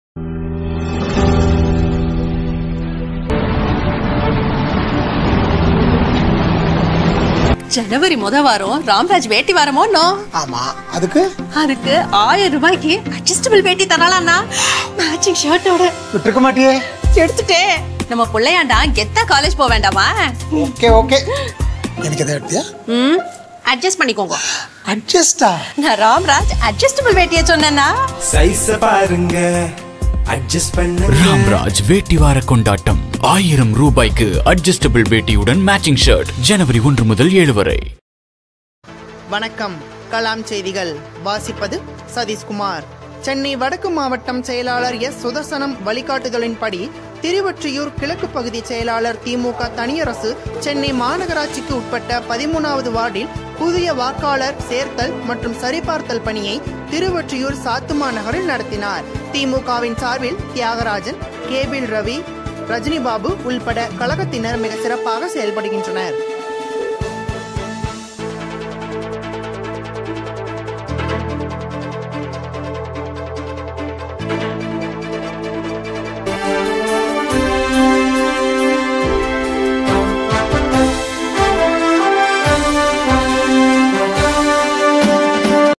KALAM NEWS TV சென்னை வடக்கு மாவட்டம் செயலாளர்.எஸ்..சுதர்சனம் வழிகாட்டுதலின் படியும்,திருவொற்றியூர் க
KALAM NEWS TV
சென்னை வடக்கு மாவட்டம் செயலாளர்.எஸ்..சுதர்சனம் வழிகாட்டுதலின் படியும்,திருவொற்றியூர் கிழக்கு பகுதி செயலாளர்  தி.மு.தனியரசு   சென்னை மாநகராட்சிக்கு உட்பட்ட 13வது வார்டில் புதிய வாக்காளர் சேர்த்தல் சரிபார்த்தல் பணியை திருவொற்றியூர் சாத்துமா நகர் நடைபெறுகிறது